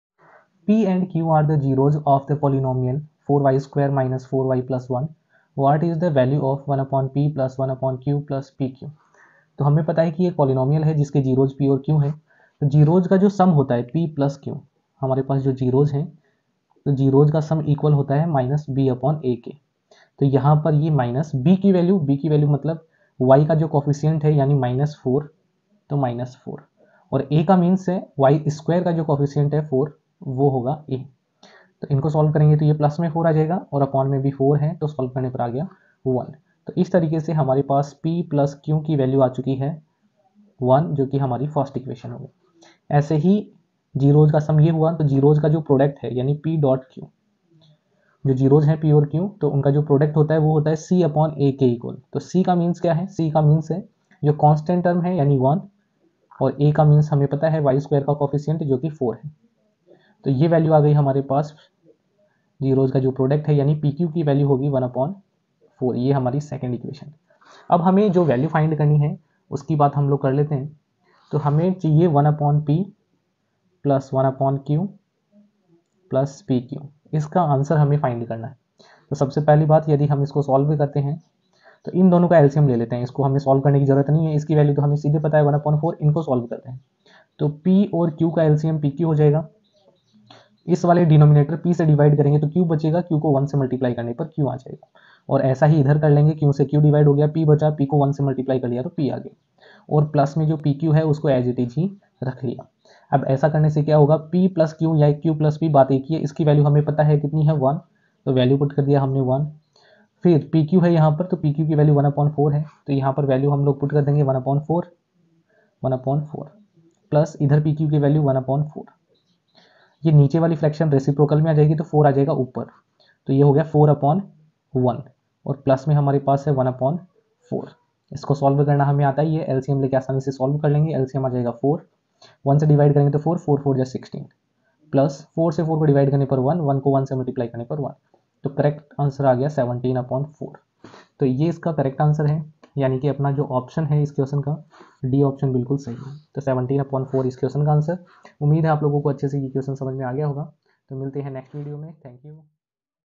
p and q are the zeroes of the polynomial 4y^2-4y+1. What is the value of 1/p + 1/q +pq? || CBSE Que.
p and q are the zeroes of the polynomial 4y^2-4y+1. What is the value of 1/p + 1/q +pq?

CBSE
Quadratic polynomial question
maths question